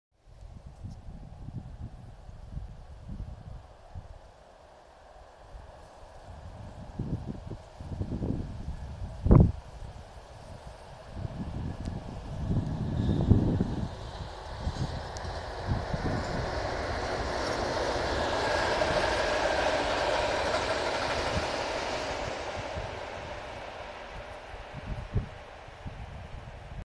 ET22 - 1035 z mieszanym składem oraz z SM 48 - 052. 22.08.2019
Lokomotywa ET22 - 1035 "Byk" z mieszanym składem 3 wagonów oraz z lokomotywą spalinową SM 48 - 052 przejeżdża przez Sędziszów Małopolski. Film nagrany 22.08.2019. Miłego oglądania !